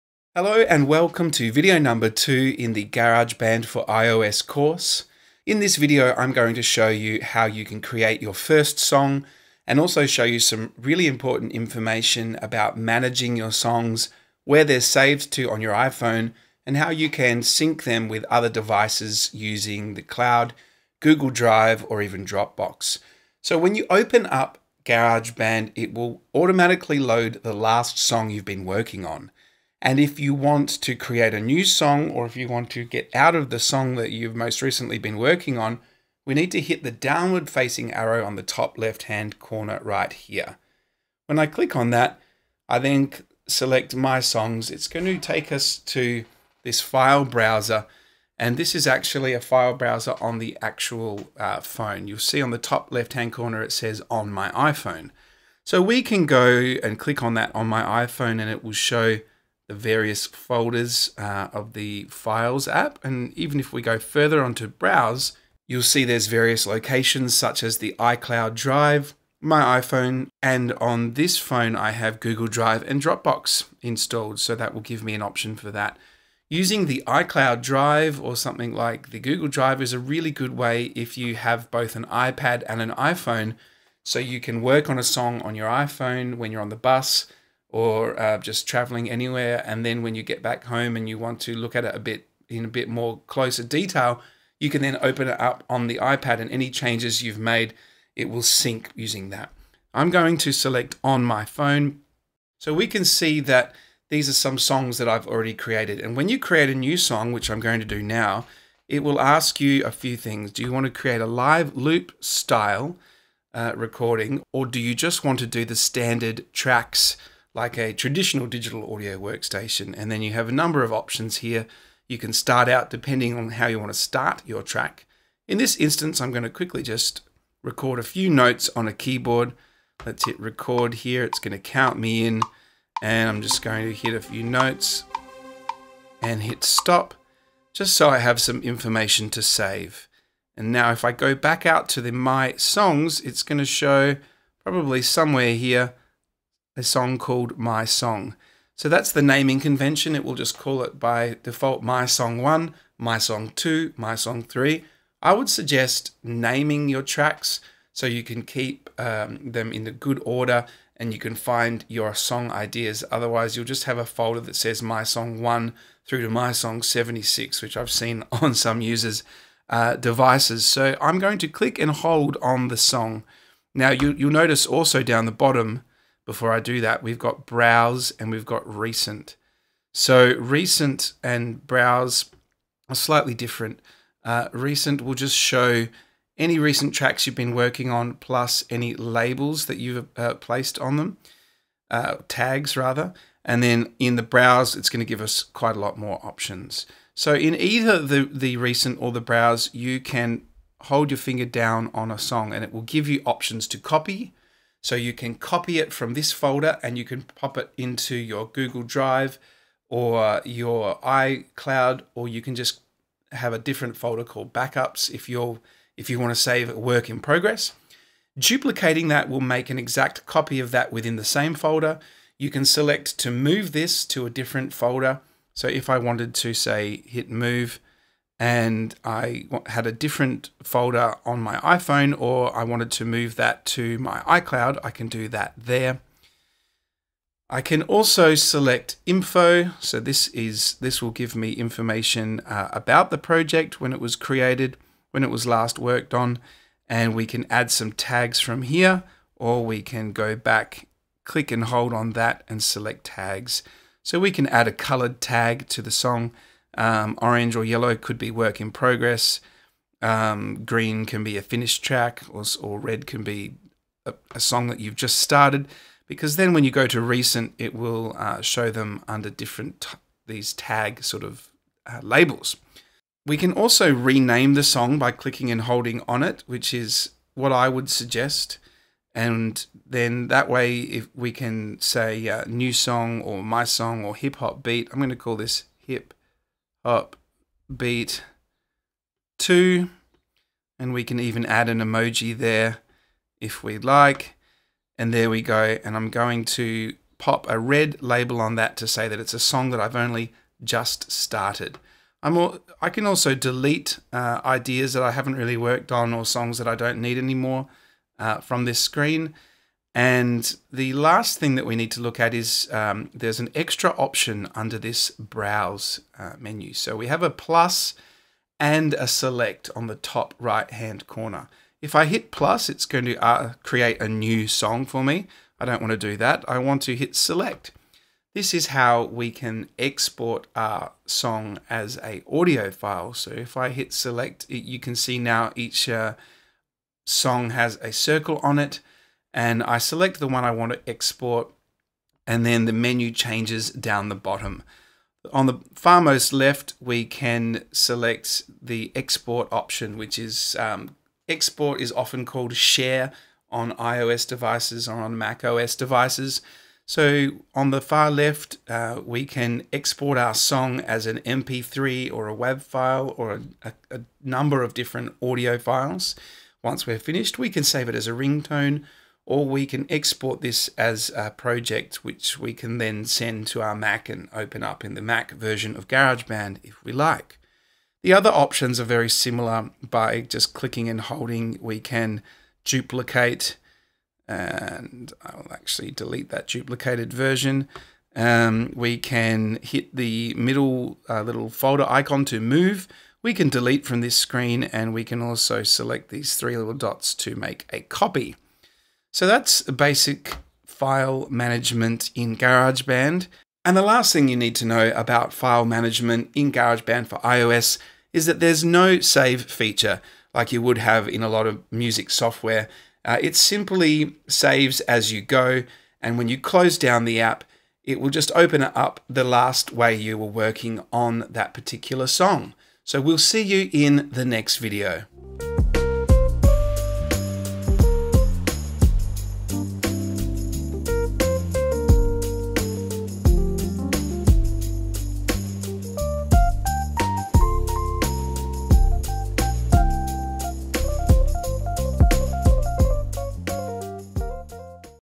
iPhone GarageBand Course #2 [Your First Song]
This is my GarageBand iPhone Tutorial series which is a free course where you can learn how to use GarageBand for iOS on your iPhone and iPad. GarageBand is free music production software for all iPhone, iPad and Mac OS X users.

I have had many students ask me to make a full course exploring all of the features of the iPhone and iPad versions of Garage Band. It's time to have some fun and make music GarageBand iPhone.

In this second video I will show you how to make your first song and where the files get saved on your phone.